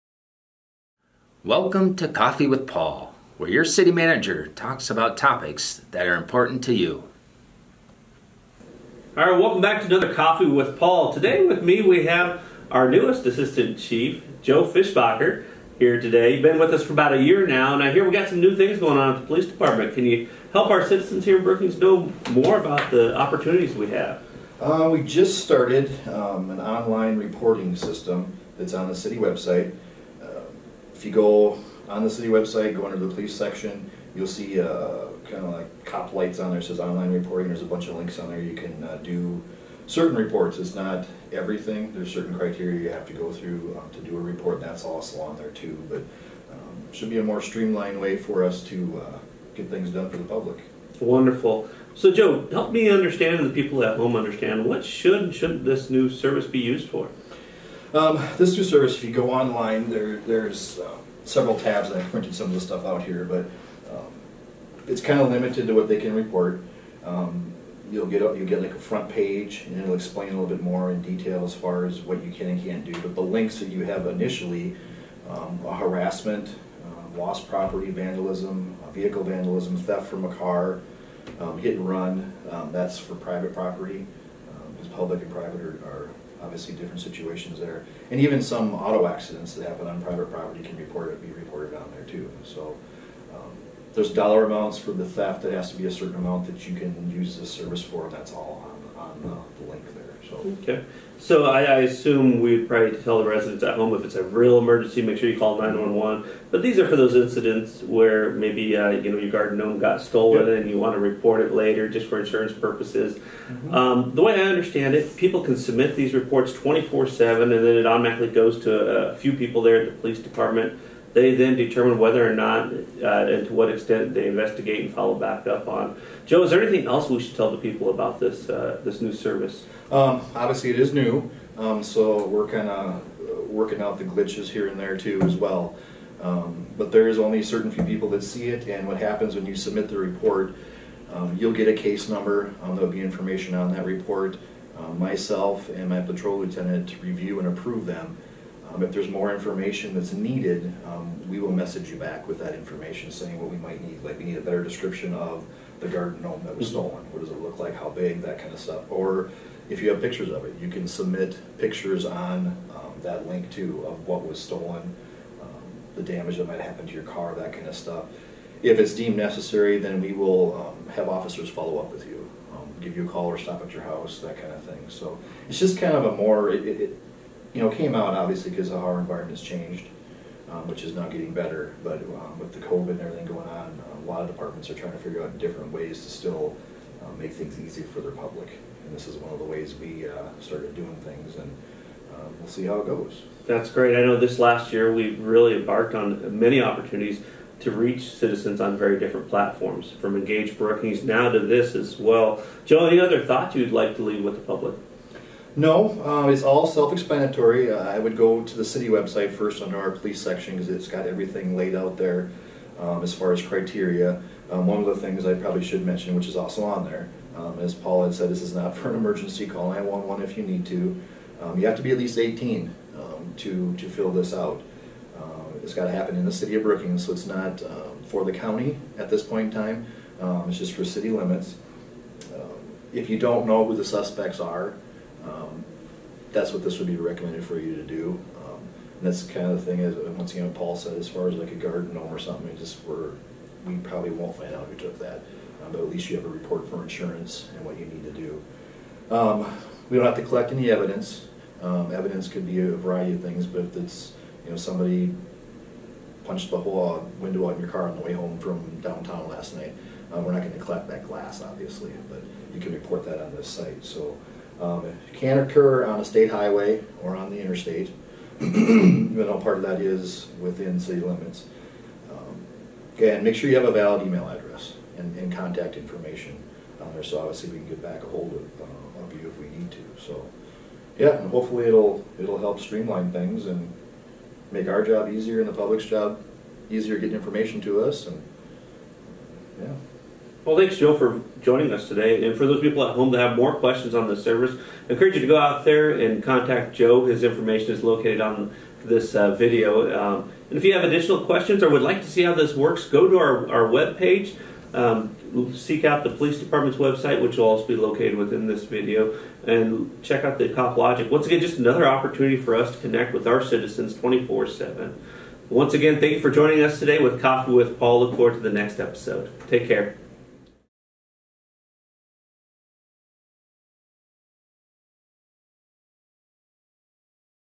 Coffee with Paul Episode 19 | August 20, 2021
Do you want to know more about City operations -- how about what the city manager does? Are you curious about what’s going on in Brookings right now? In our video broadcast Coffee with Paul, Brookings City Manager Paul Briseno chats about topics that matter to you and the Brookings community.

Special Guest: Assistant Police Chief, Joe Fishbaugher talks about the new Online Police Reporting System that residents can utilize to submit non-emergency related police reports like auto accidents, theft, lost property and more.

VIDEO RESOURCES AND LINKS